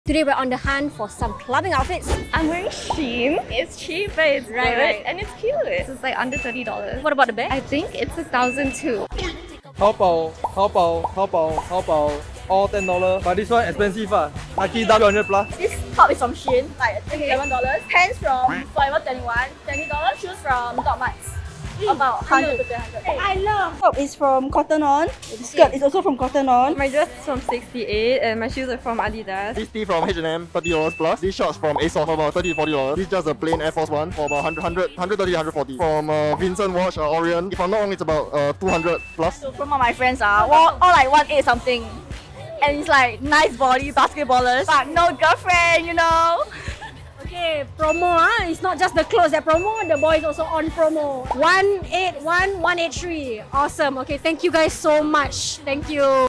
Clubbing outfits at Zouk | ZULA Spotted
We’re on the hunt for some clubbing outfits in Singapore… 👀